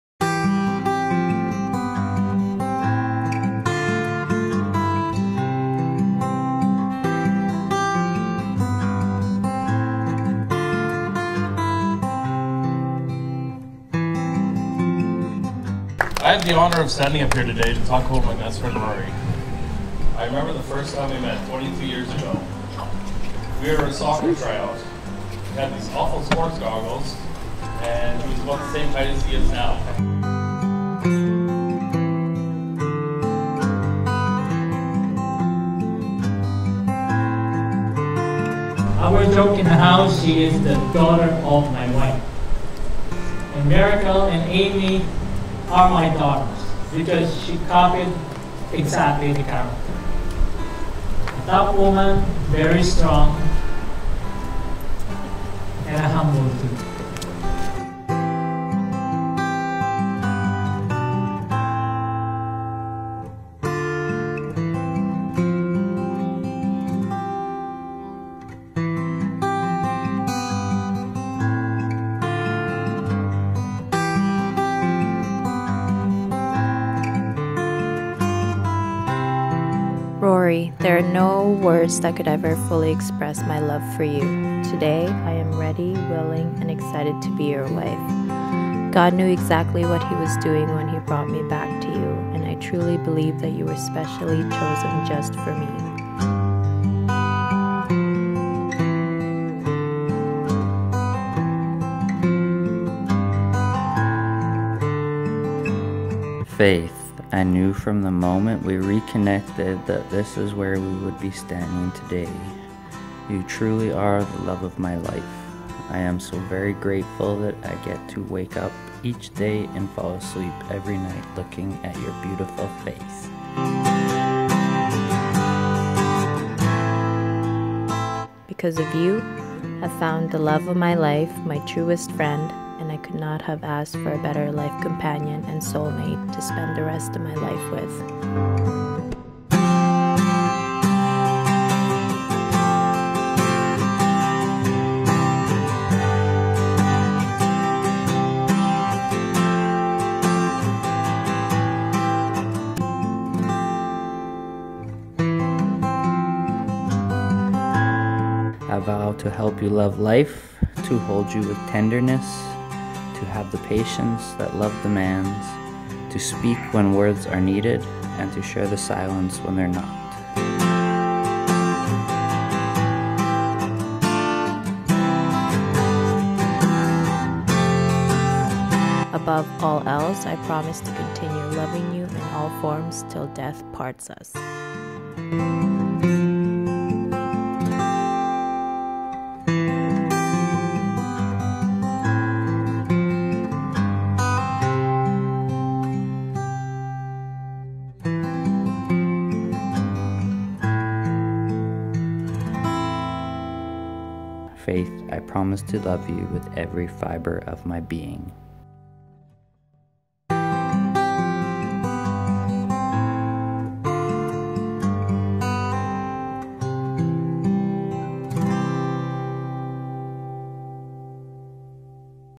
Faith & Rory's Wedding ○ Majestic Colonial Punta Cana
Got the opportunity to film and edit my first wedding, with the help of my amazing girlfriend! Faith and Rory had a gorgeous wedding at the Majestic Colonial Resort in Punta Cana!

Be completely humble and gentle; be patient, bearing with one another in love. Ephesians 4:2

Filmed on:
○ Panasonic GH3
○ Samsung S9
○ Mavic Pro

Edited on:
○ Premiere Pro

Many Edit tweaks by:
○ Boods